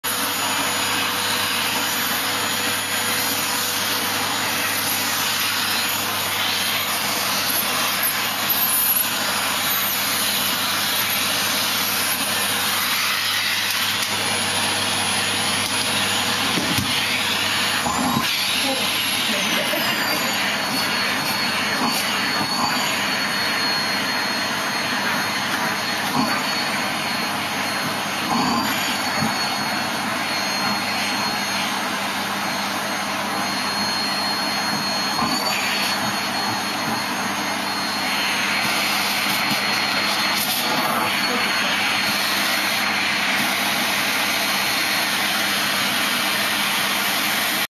掃除機が好きな猫
小春（元♂）です。
真冬のスキー場で拾いました。

普通猫は掃除機が嫌いなのですが、掃除機をかけると寄ってきます。
抜け毛を吸い取るのが日課になりました。